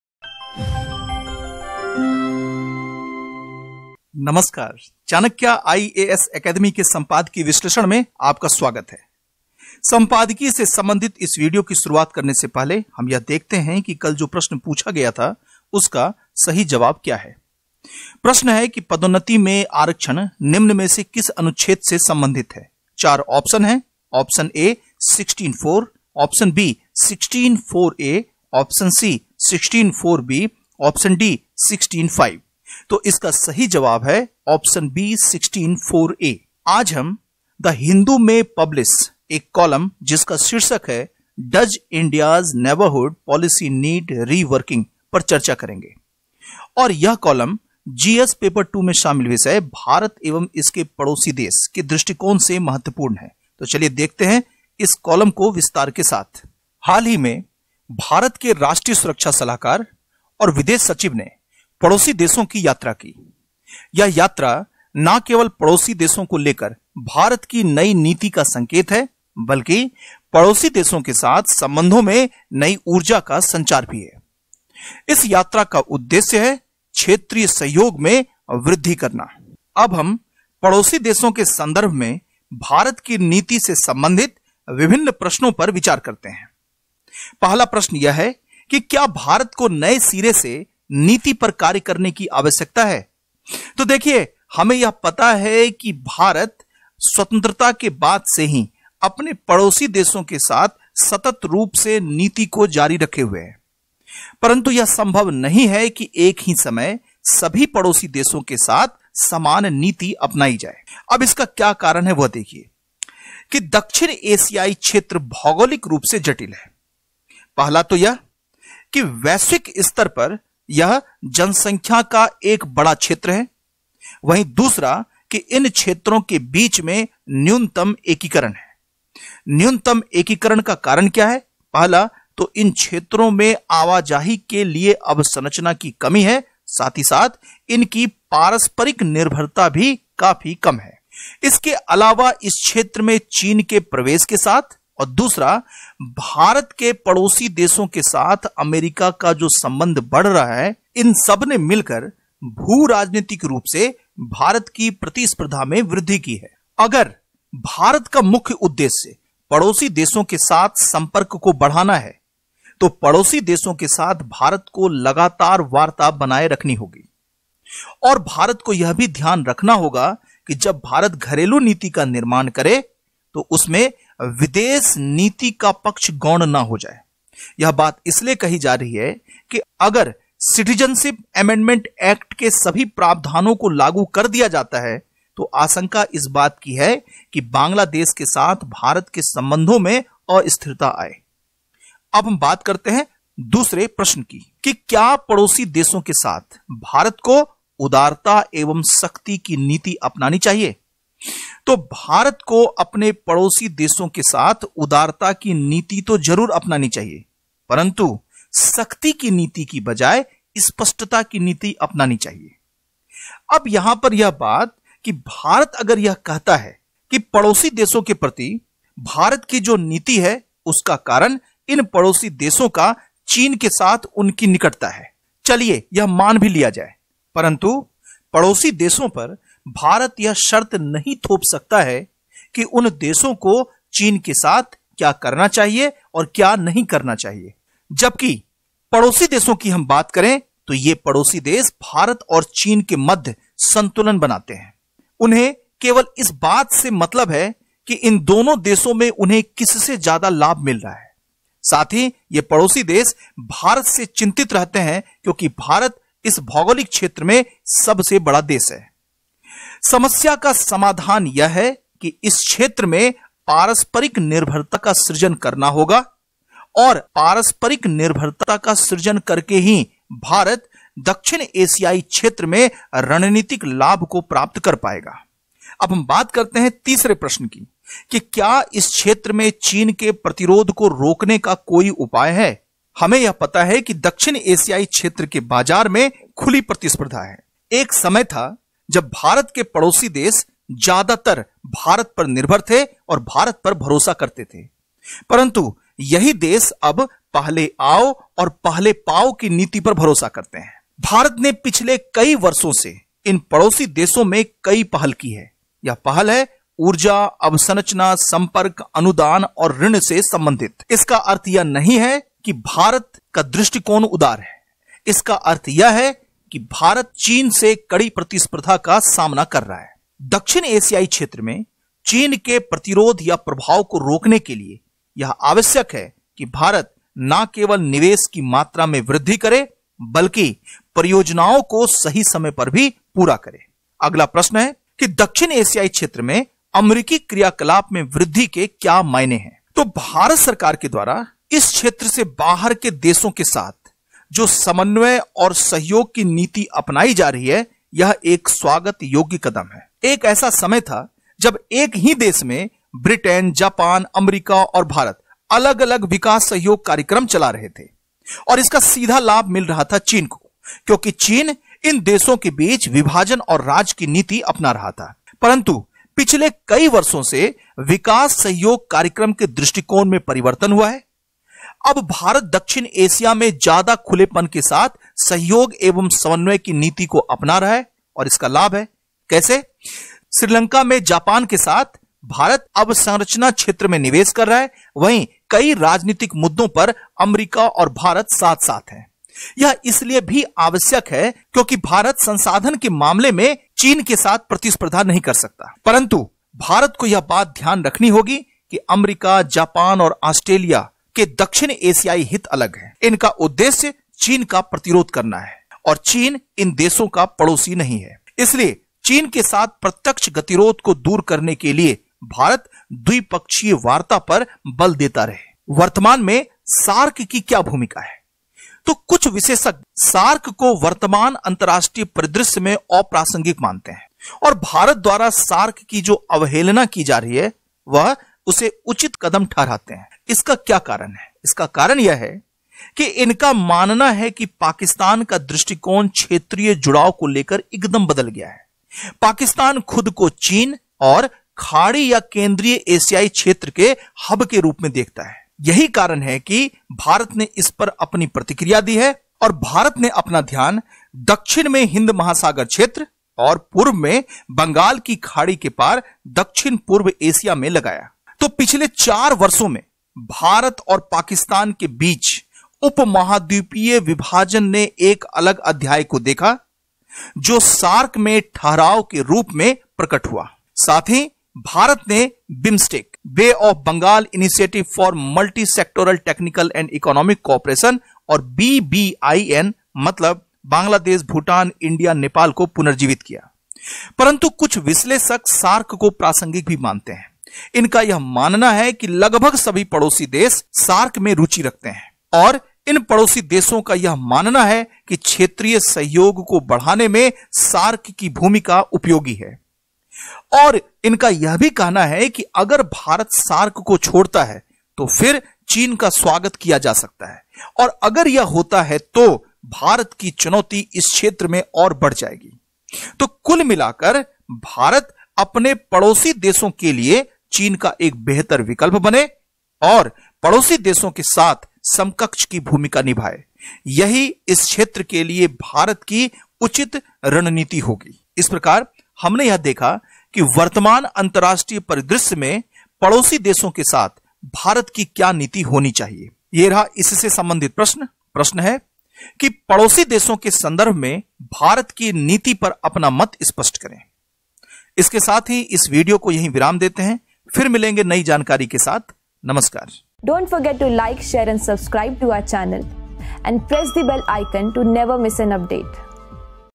संपादकीय विश्लेषण | Chanakya's Daily Editorial Analysis in Hindi for IAS/PCS/UPSC Exams | 04.12.2020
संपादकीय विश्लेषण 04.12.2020

Chanakya's Daily Editorial Analysis in Hindi for IAS/PCS/UPSC Exams presented by प्रियंवद प्रज्ञेश